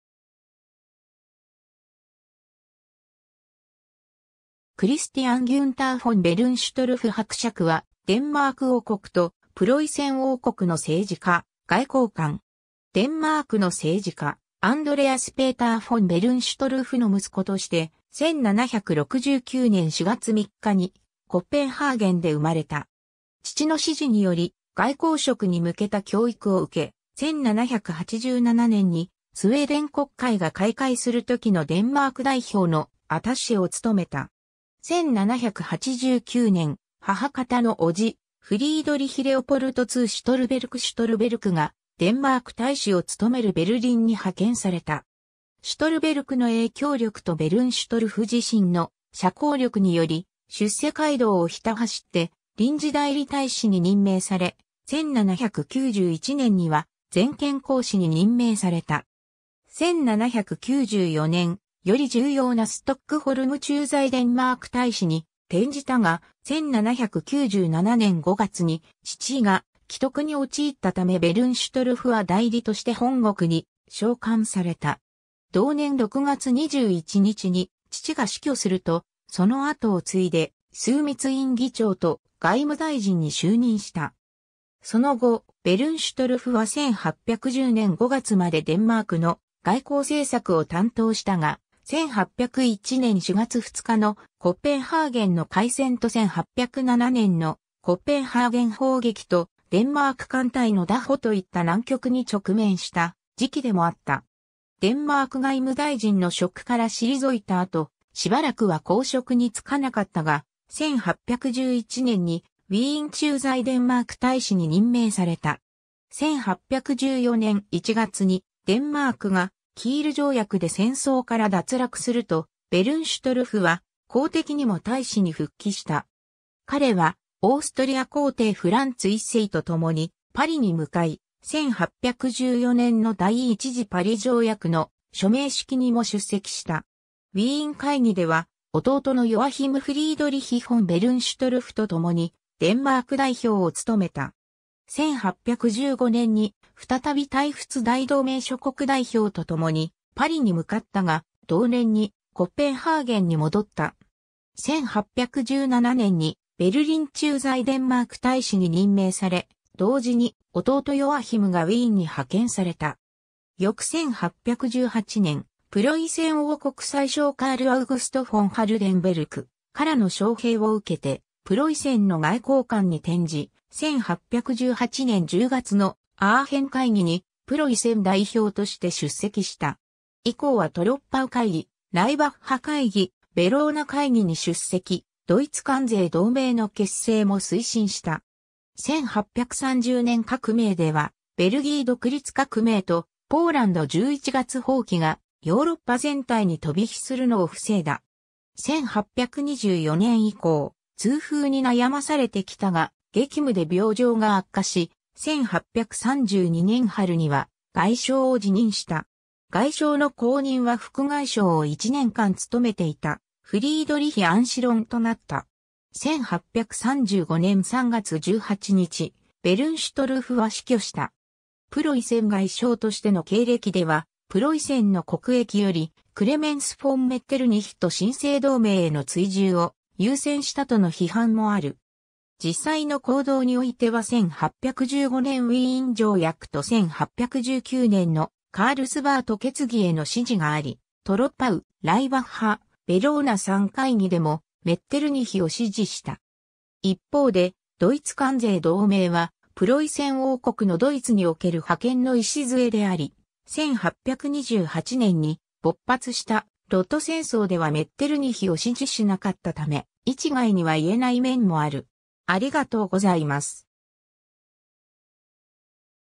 クリスティアン・ギュンター・フォン・ベルンシュトルフ
デンマーク＝ノルウェーの政治家
クリスティアン・ギュンター・フォン・ベルンシュトルフ伯爵（ドイツ語: Christian Günther Graf von Bernstorff、1769年4月3日 - 1835年3月18日）は、デンマーク王国とプロイセン王国の政治家、外交官。
 デンマークの政治家アンドレアス・ペーター・フォン・ベルンシュトルフの息子として、1769年4月3日にコペンハーゲンで生まれた。
父の指示により外交職に向けた教育を受け、1787年にスウェーデン国会が開会するときのデンマーク代表のアタッシェを務めた。
1789年、母方のおじフリードリヒ・レオポルト・ツー・シュトルベルク＝シュトルベルクがデンマーク大使を務めるベルリンに派遣された。
シュトルベルクの影響力とベルンシュトルフ自身の社交力により出世街道をひた走って臨時代理大使に任命され、1791年には全権公使に任命された。
 1794年、より重要なストックホルム駐在デンマーク大使に転じたが、1797年5月に父が危篤に陥ったためベルンシュトルフは代理として本国に召還された。
同年6月21日に父が死去すると、その後を継いで枢密院議長（当時のデンマーク首相にあたる）と外務大臣に就任した。
その後、ベルンシュトルフは1810年5月までデンマークの外交政策を担当したが、1801年4月2日のコペンハーゲンの海戦と1807年のコペンハーゲン砲撃とデンマーク艦隊の拿捕といった難局に直面した時期でもあった。
 デンマーク外務大臣の職から退いた後、しばらくは公職につかなかったが、1811年に（デンマークとオーストリアが名目上戦争状態にあったにもかかわらず）ウィーン駐在デンマーク大使に任命された。
1814年1月にデンマークがキール条約で戦争から脱落すると、ベルンシュトルフは公的にも大使に復帰した。
彼はオーストリア皇帝フランツ1世とともにパリに向かい、1814年の第一次パリ条約の署名式にも出席した。
ウィーン会議では弟のヨアヒム・フリードリヒ・フォン・ベルンシュトルフとともにデンマーク代表を務めた。
1815年に再び対仏大同盟諸国代表とともにパリに向かったが、同年にコペンハーゲンに戻った。
1817年にベルリン駐在デンマーク大使に任命され、同時に弟ヨアヒムがウィーンに派遣された。
 翌1818年、プロイセン王国宰相カール・アウグスト・フォン・ハルデンベルクからの招聘を受けてプロイセンの外交官に転じ、1818年10月のアーヘン会議にプロイセン代表として出席した。
以降はトロッパウ会議、ライバッハ会議、ヴェローナ会議に出席、ドイツ関税同盟の結成も推進した。
1830年革命ではベルギー独立革命とポーランド11月蜂起がヨーロッパ全体に飛び火するのを防いだ。
 1824年以降、痛風に悩まされてきたが、激務で病状が悪化し、1832年春には外相を辞任した。
外相の後任は副外相を1年間務めていたフリードリヒ・アンシロンとなった。
1835年3月18日、ベルンシュトルフは死去した。
 プロイセン外相としての経歴ではプロイセンの国益よりクレメンス・フォン・メッテルニヒと神聖同盟への追従を優先したとの批判もある。
実際の行動においては1815年ウィーン条約と1819年のカールスバート決議への支持があり、トロッパウ、ライバッハ、ヴェローナ3会議でもメッテルニヒを支持した。
一方でドイツ関税同盟はプロイセン王国のドイツにおける覇権の礎であり、1828年に勃発した露土戦争ではメッテルニヒを支持しなかったため、一概には言えない面もある。